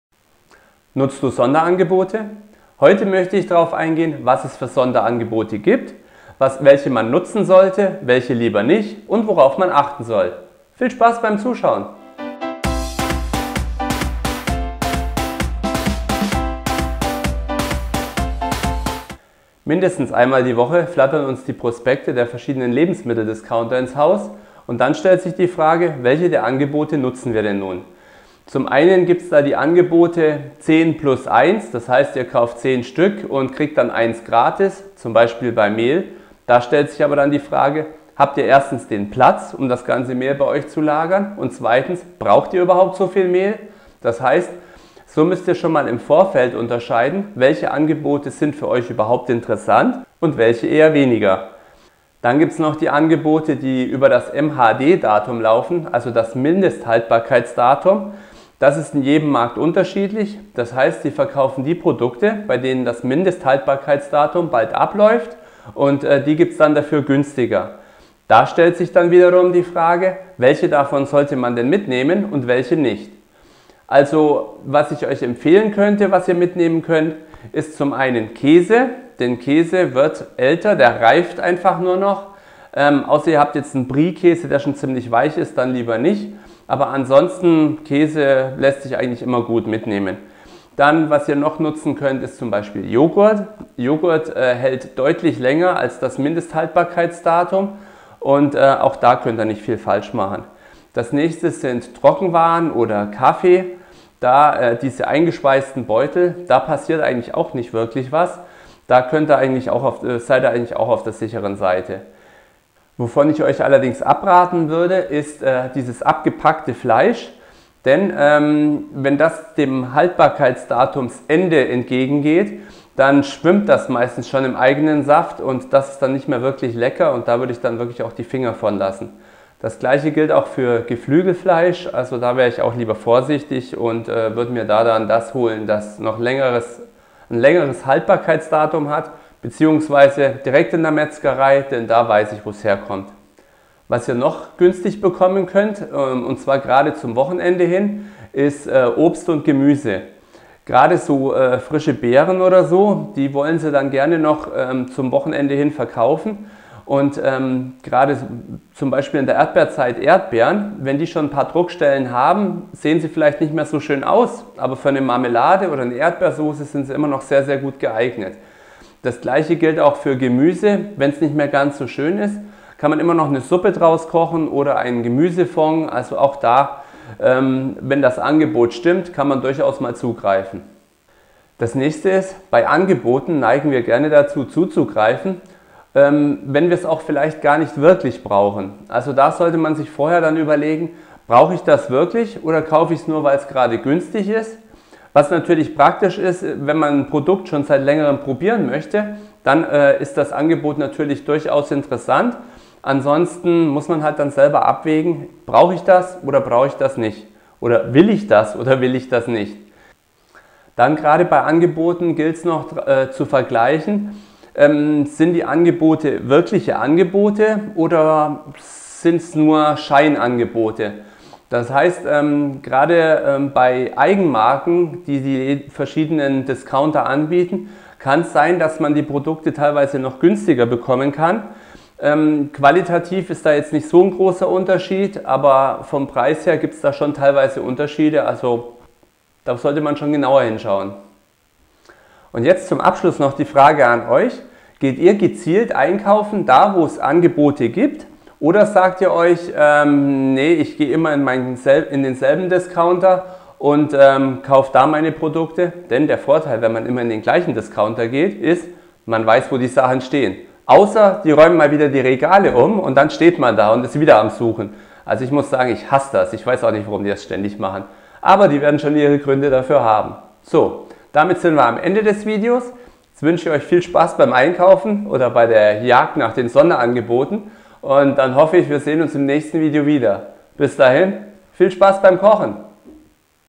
Sonderangebote nutzen, aber richtig - Sparen beim Einkaufen
Wer beim Einkaufen sparen möchte, für den habe ich hier ein paar Tipps. Es gibt verschieden Sonderangebote, doch welche rentieren sich und welche kann man liegen lassen. Welche MHD Ware kann man kaufen und welche besser nicht?
Im Video gibt es Tipps fürs sparen beim Einkauf!

Regie und Produktion: Markus Meier / Meiers Kochtipps / Mietkoch

Meine Ausrüstung:
*Sony a 58
*Walimex Objektiv
*Panasonic Lumix

*Durch den Klick auf diese Links unterstützt Du bei einem Kauf meinen Youtube-Kanal.